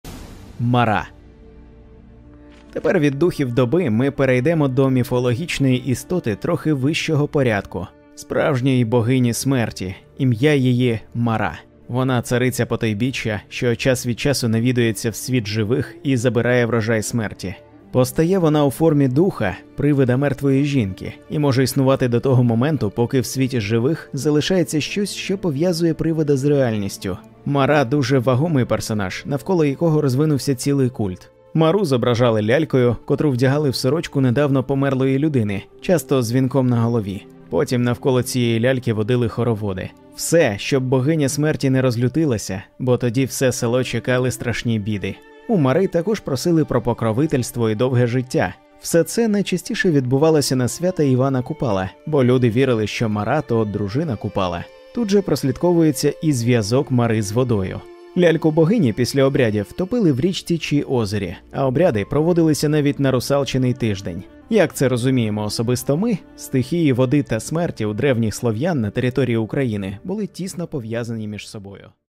Нічниця Україна містична
В Цьом відео ві дізнаєтесь про одну з містичних створить яку  називали наши предки Нічниця.Тож хто вона и чого її так боялися!!